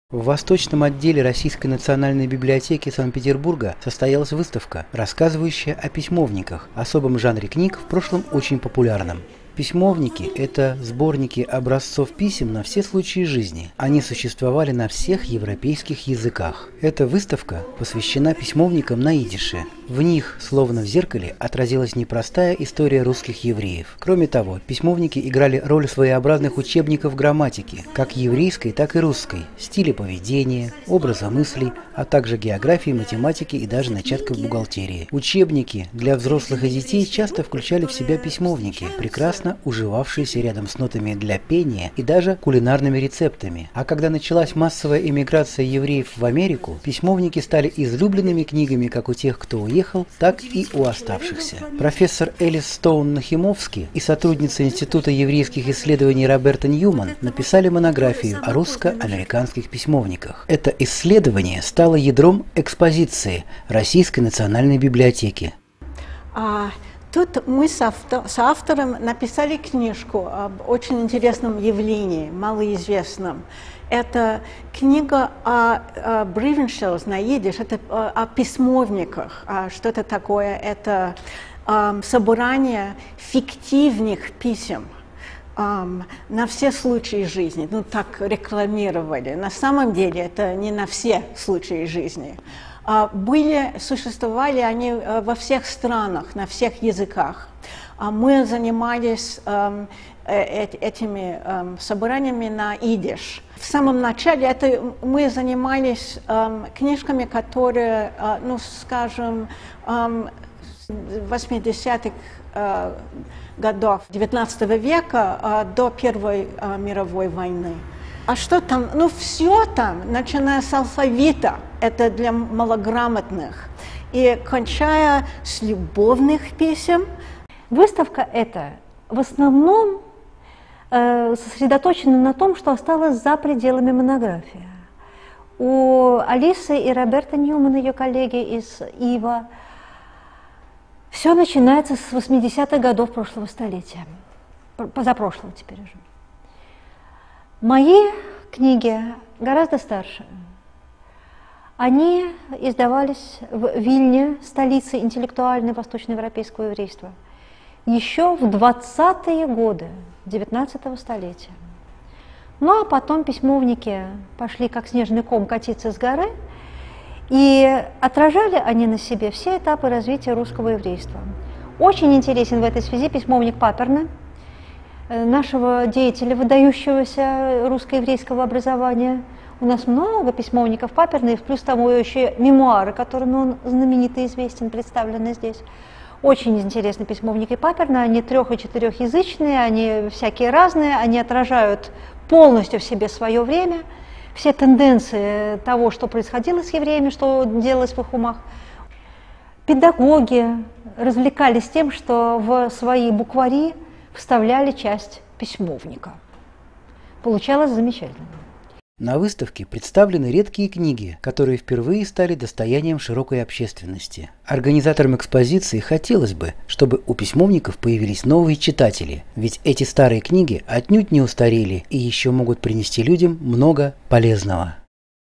«Я к Вам пишу…»
Выставка проходила в сентябре 2015 года в Отделе литературы стран Азии и Африки (Литейный пр.).